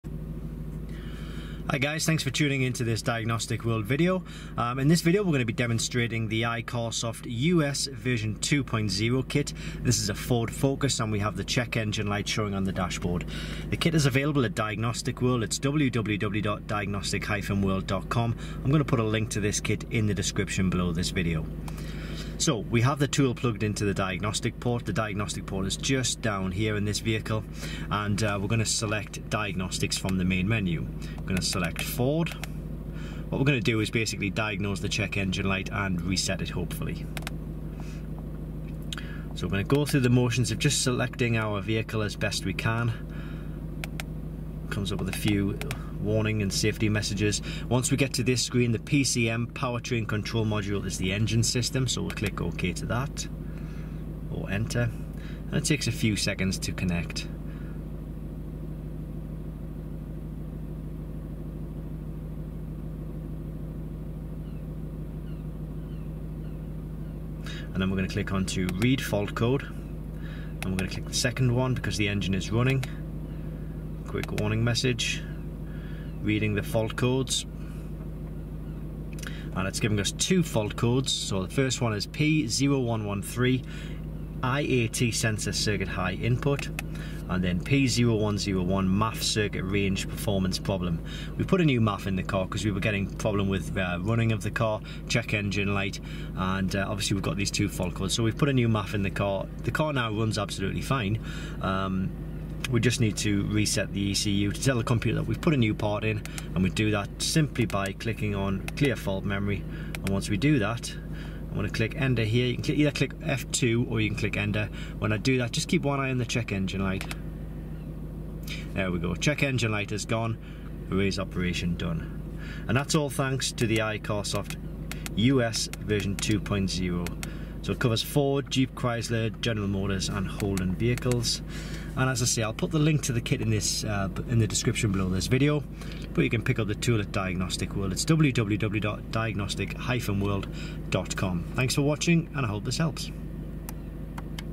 iCarsoft US v2.0 Official Demo Ford Check Engine Light Reset P0113 P0101 Scan Tool Reset
The US v2.0 is one of the best Ford code reader, scan tool or OBD2 diagnostic tool available to buy. It covers all of the main systems like engine, abs, airbags, transmission, service reset and a lot more.